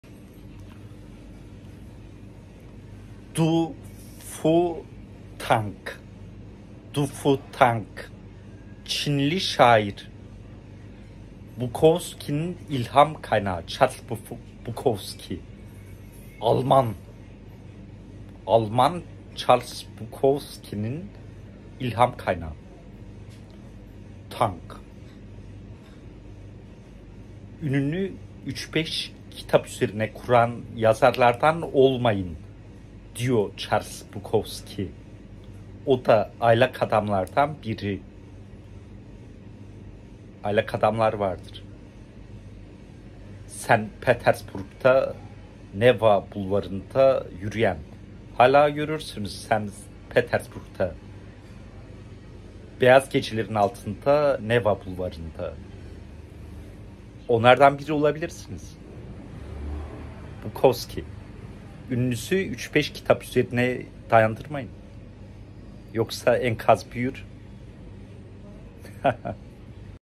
Bukowski Ününü Neye Dayandırır? Prof. Dr. Metin Bal
10 Kasım 2023 Cuma, Octopus Book Cafe, Antalya.

Su Fu Tang, Charles Bukowski’nin ılham kaynağı Çinli şair.